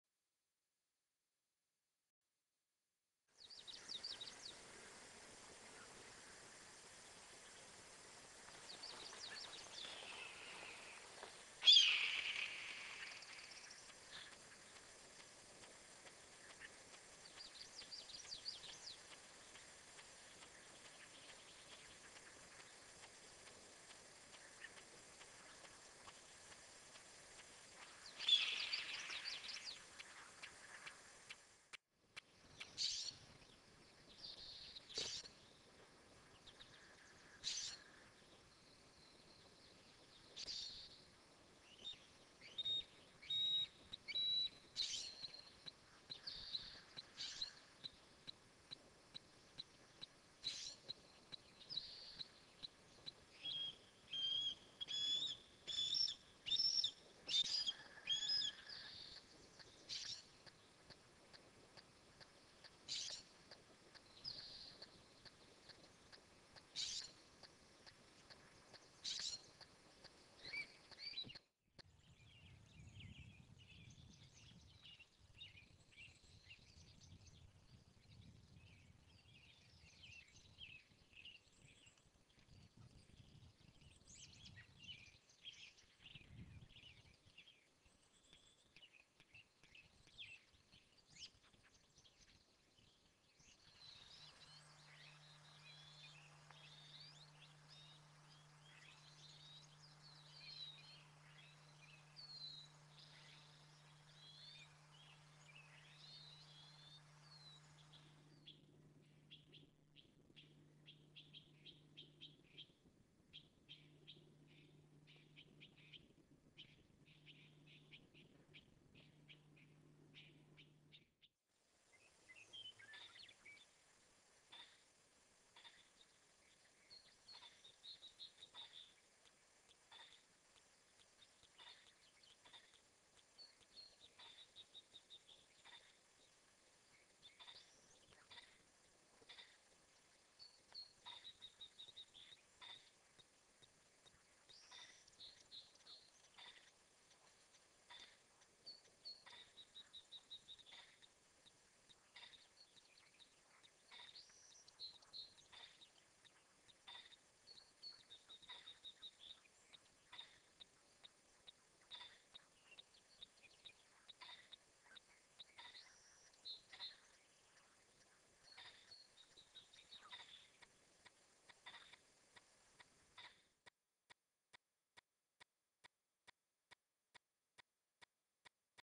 Red-tailed Hawk
Western subspecies (Buteo jamaicensis calurus) in the Willamette Valley, Oregon.  Hear the "scream" call and the whistled call. Adults and juveniles shown.